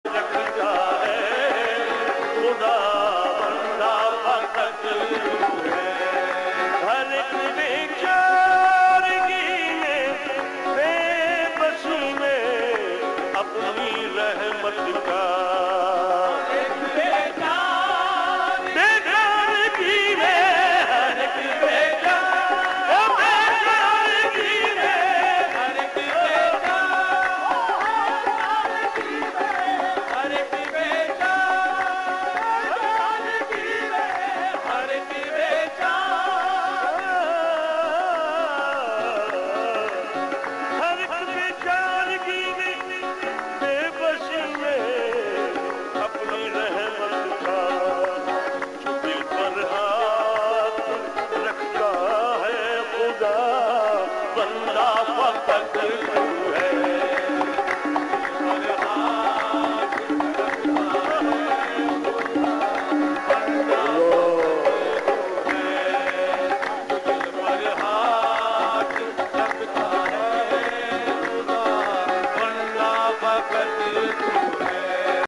Sabri Brothers - Maqbool Ahmed Sabri Crying While Remembering Ghulam Farid Sabri (MUST WATCH)
A Very Heart Touching Clip Of Haji Maqbool Ahmed Sabri (rh) Crying While Remembering His Elder Brother Haji Ghulam Farid Sabri (rh) while performing qawwali